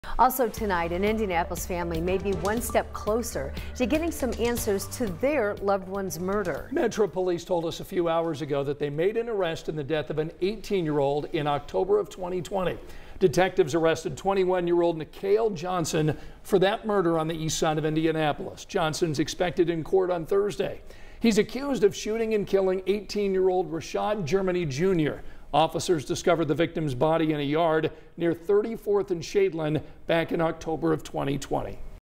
IMPD makes arrest in 2020 shooting
IMPD said Tuesday officers made an arrest in the death of an 18-year-old in October 2020.